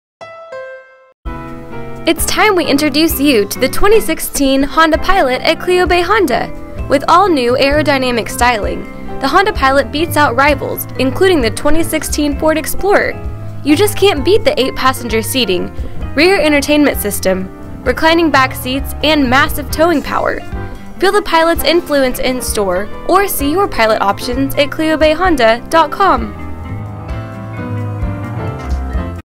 2016 Honda Pilot vs. 2016 Ford Explorer
Lucky you! You must be shopping for brand new SUVs and found yourself here at the Cleo Bay Honda blog. What makes you so lucky, you ask? We just happened to be comparing two rival SUVs today: the 2016 Honda Pilot and the 2016 Ford Explorer in Killeen, Texas. Leave the research to us. By the end of this comparison, we hope you'll see which option is the best fit for you!